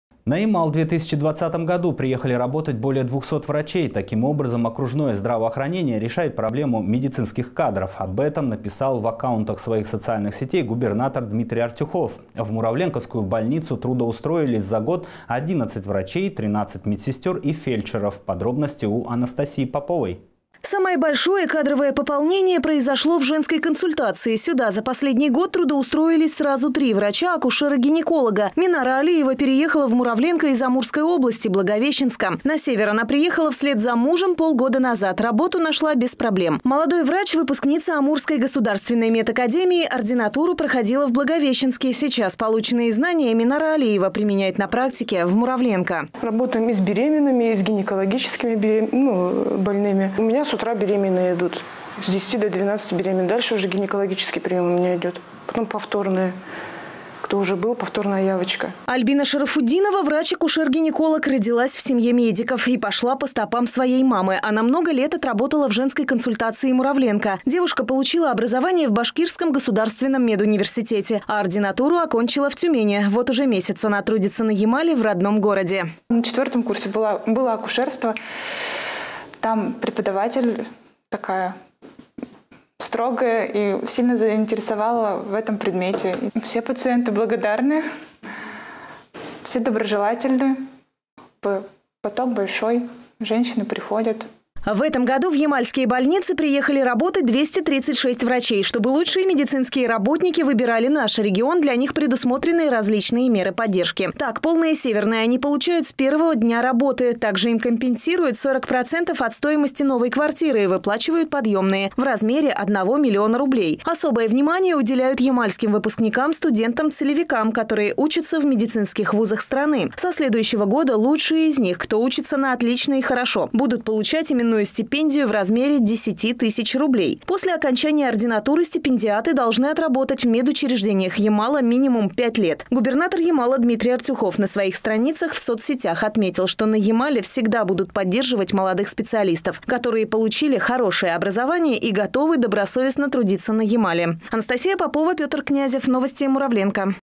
МОЛОДЫЕ ВРАЧИ ПРИХОДЯТ РАБОТАТЬ В ГОРОДСКУЮ БОЛЬНИЦУ
На Ямал в 2020 году приехали работать более двухсот врачей. Таким образом окружное здравоохранение решает проблему медицинских кадров. В Муравленковскую больницу трудоустроились за год 11 врачей, 13 медсестер и фельдшеров.